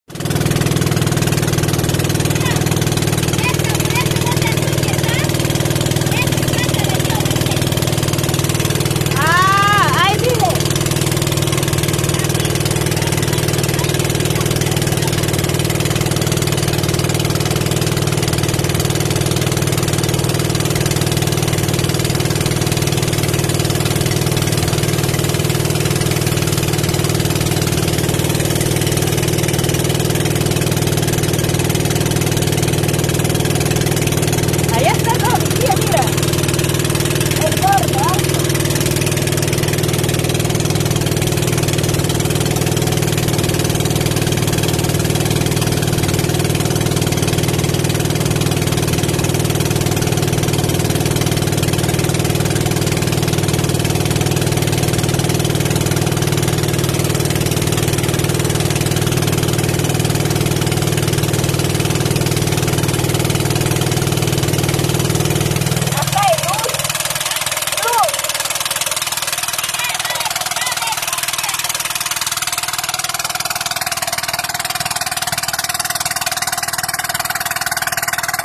rió nieva viajando que paisajes muy hermosos en amazonas peru
rió nieva viajando japaime escuela en amazonas peru